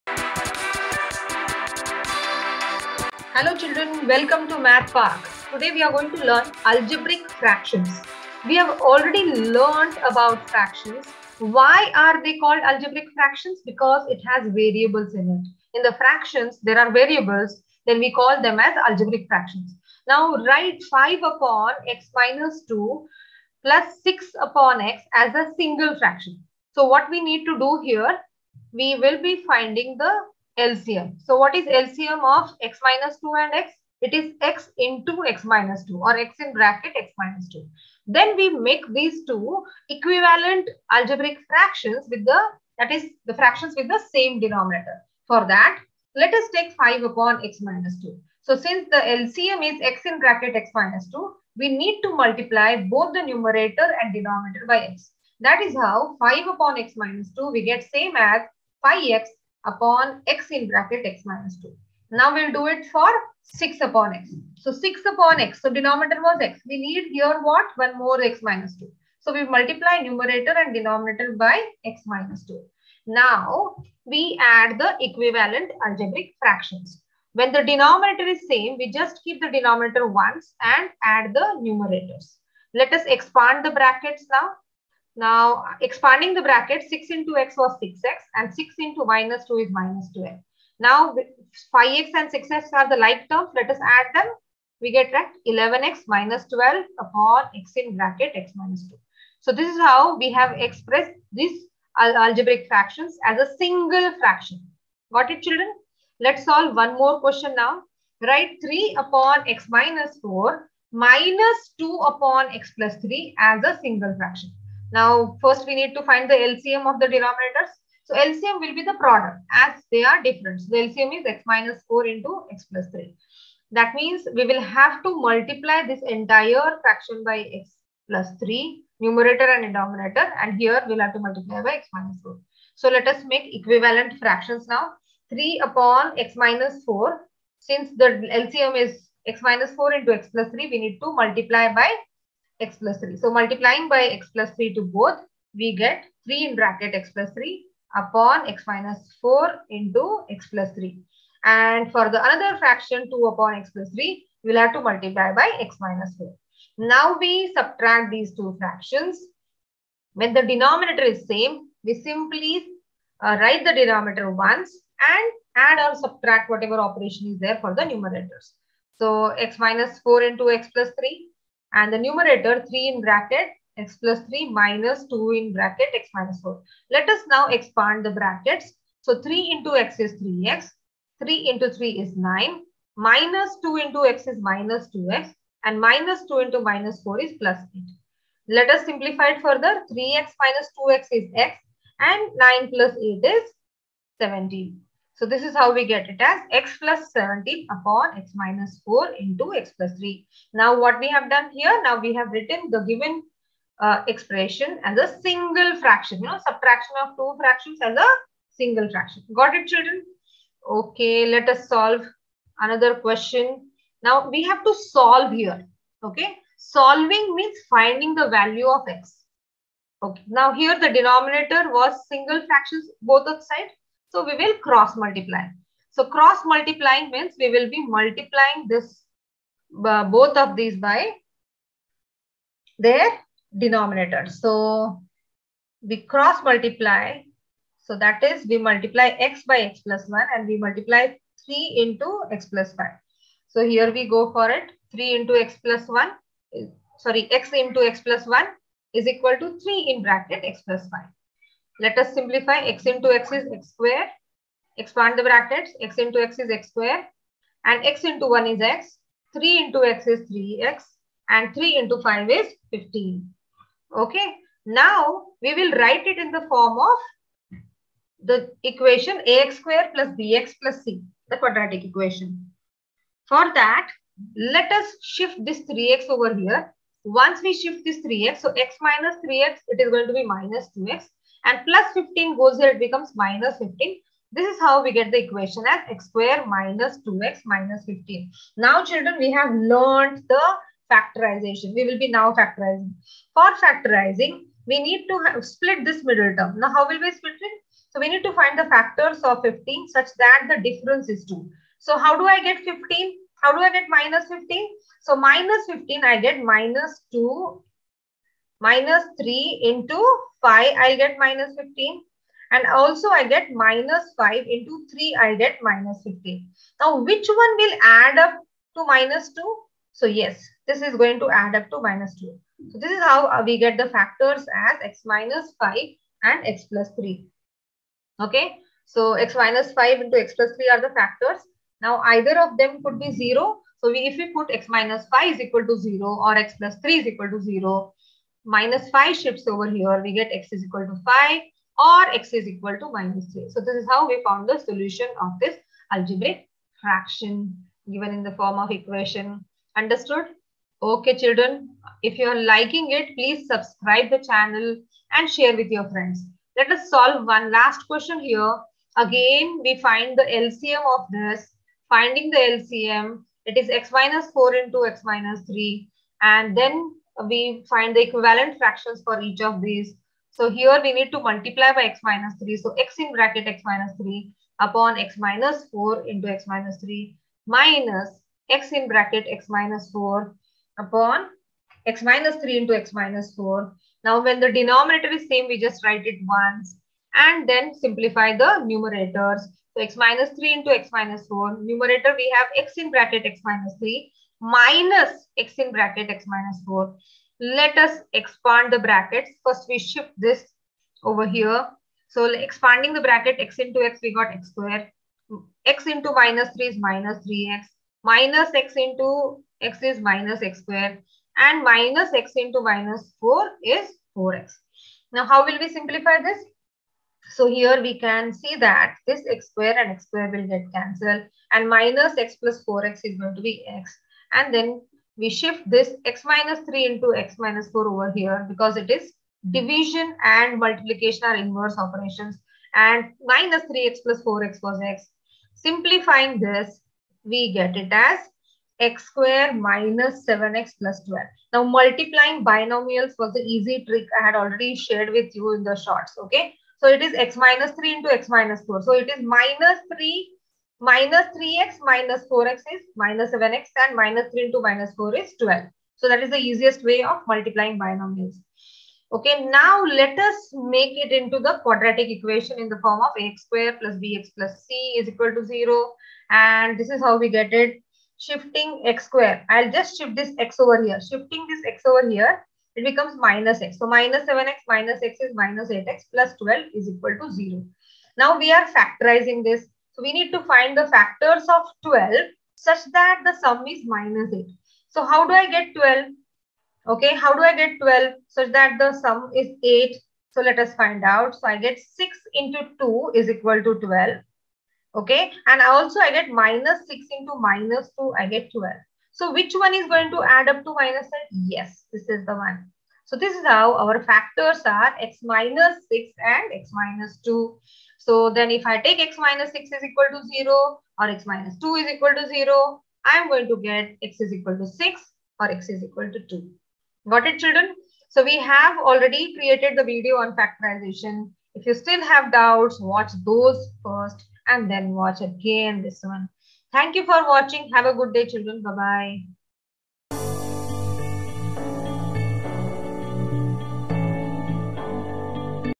Algebraic fractions|Further solving equations|IGCSE@mathpark8023
This video has simplification of Algebraic fractions Ex14. 14 Karein Morrison